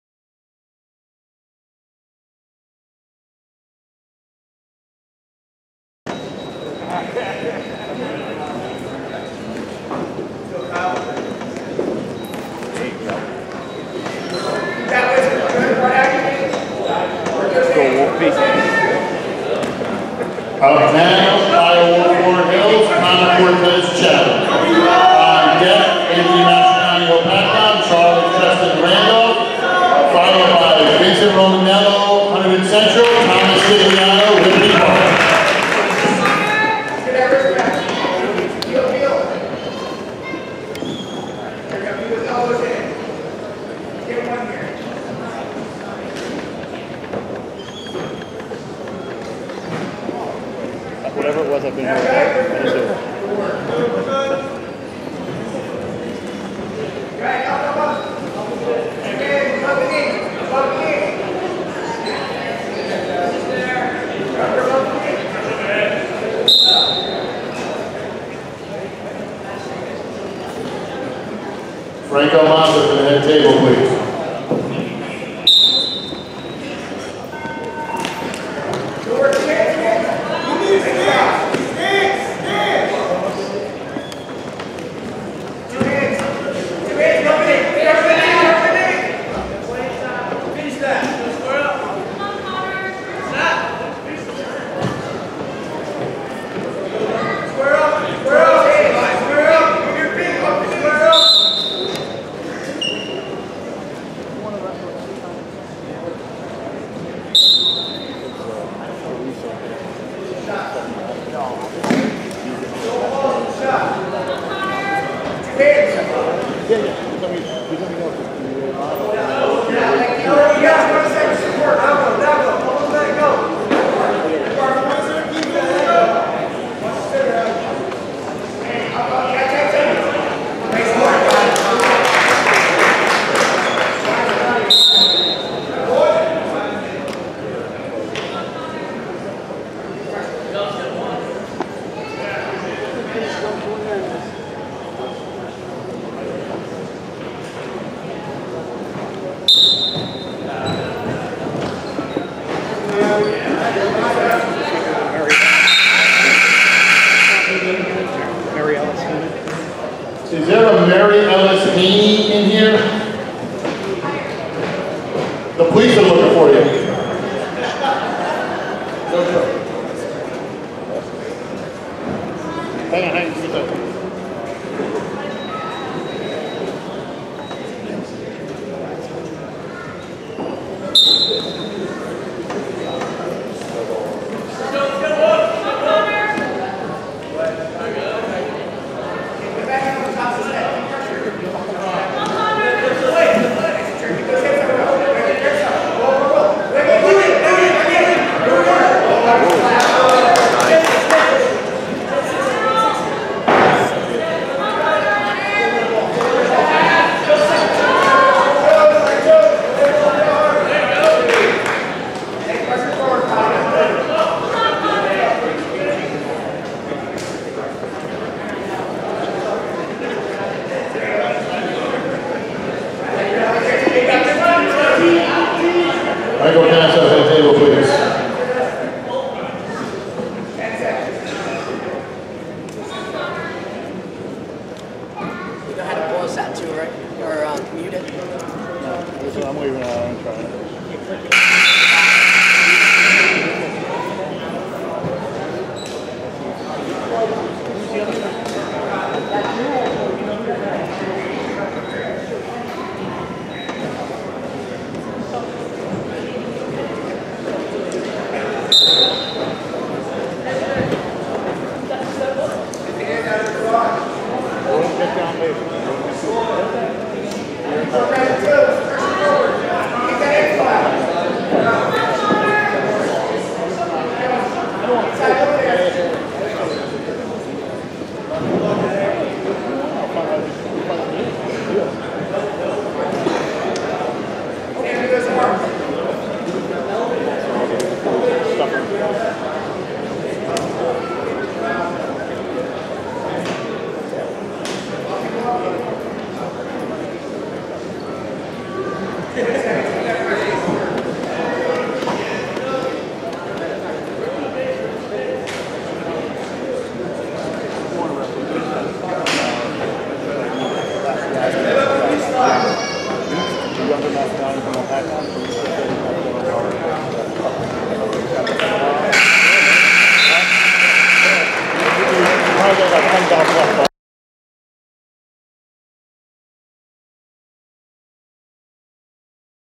6th Annual All Star Match 126 Cortese vs. Wulff
The following is a 126 bout between Connor Cortese (Chatham) and Kyle Wulff (Warren Hills) from the 6th Annual HWS vs. Morris All Star Match on May 19th at Mendham High School in Morris County, NJ.